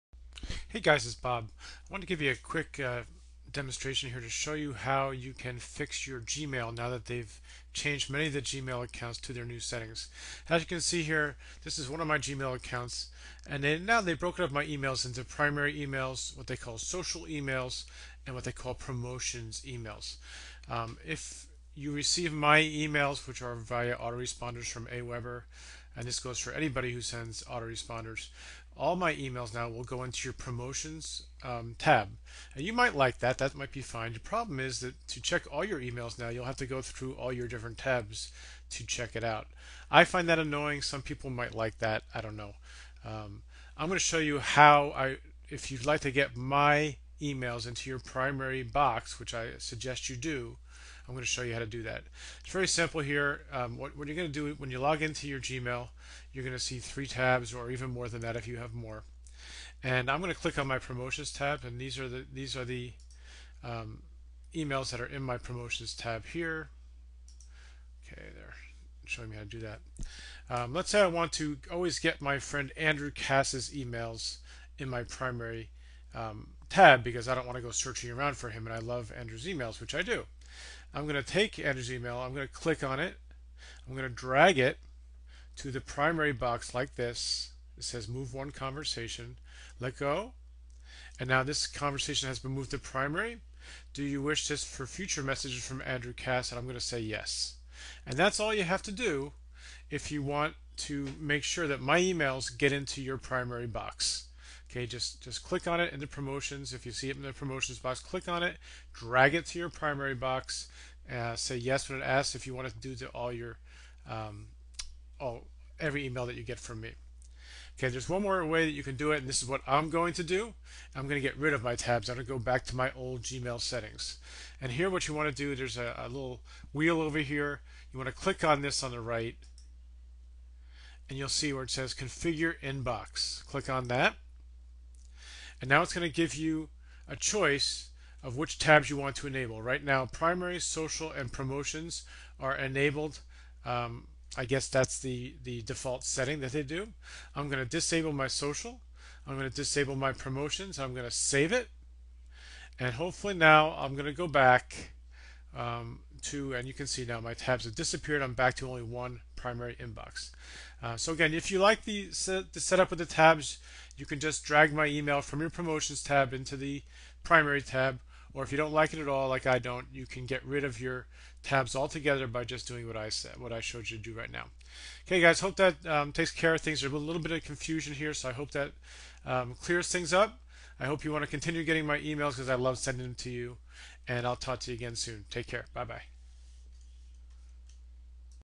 Getting Your Gmail Inbox Back To Normal Again
Gmail has begun rolling out their new Tab features to segregate emails you receive in your inbox.  Some will like it, some will not.  In this video, I show you how to move emails from your Promotions tab to your Primary tab permanently.  Also, I show you exactly how to get rid of the tab feature altogether.